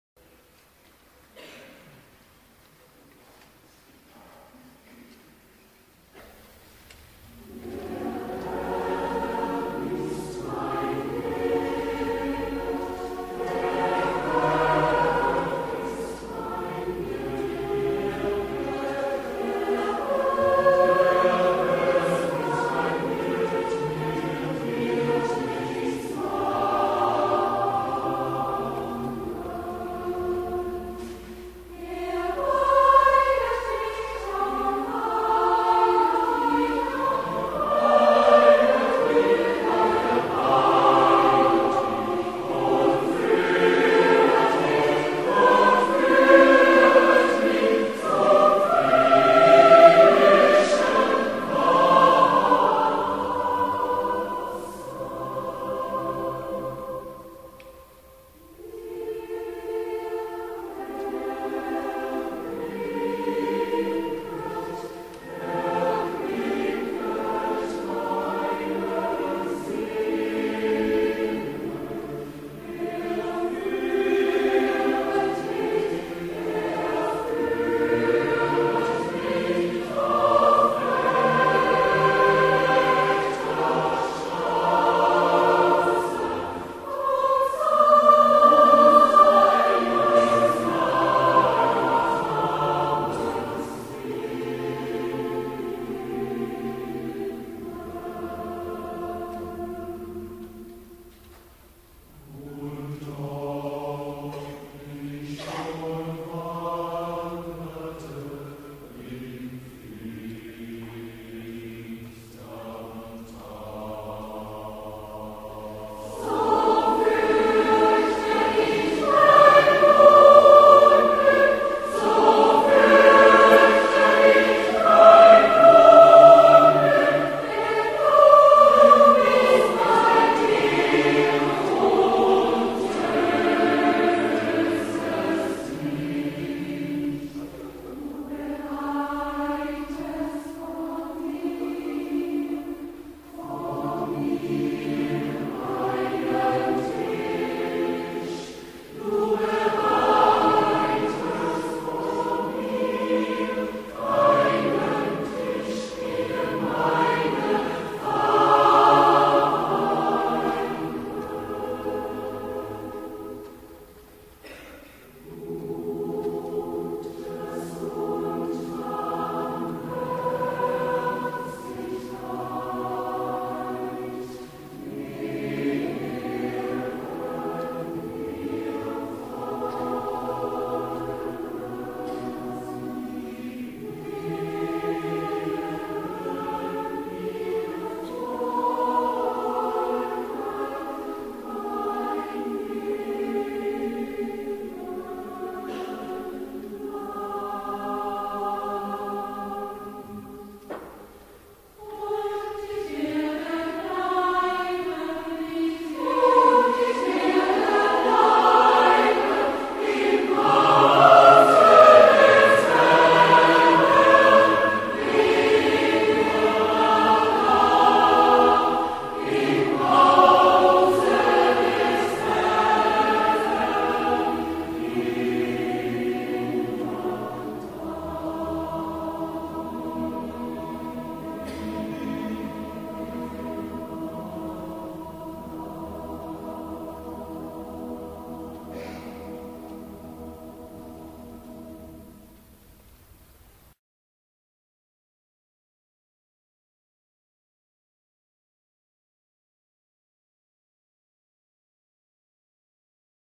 PS.23
Der 23. Psalm, auch als Hirtenpsalm oder Psalm vom guten Hirten bezeichnet, gehört zu den bekanntesten Bibeltexten. Obwohl seine Bilder in der altorientalischen Viehzüchtergesellschaft wurzeln, vermag er offensichtlich auch heutige Menschen unmittelbar anzusprechen und leiht ihnen die Worte für das Bekenntnis einer persönlichen Glaubensüberzeugung. Der Psalm ist Teil des jüdischen Psalters und ein weit verbreitetes liturgisches Gebet.